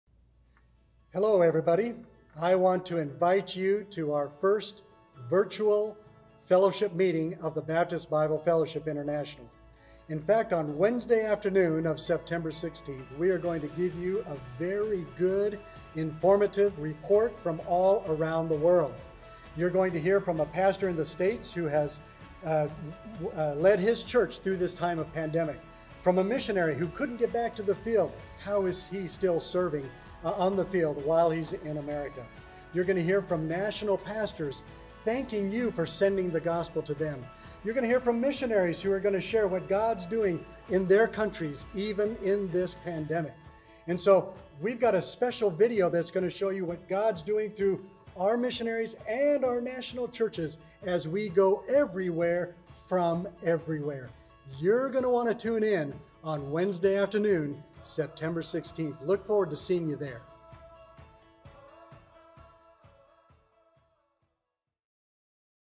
Jon Konnerup Promo for BBFI Virtual Conference 2020
BBFI Virtual Conference, September 14-16, 2020 daily from 1:00-5:00 PM (CST). Missions Emphasis is on Wednesday, September 16.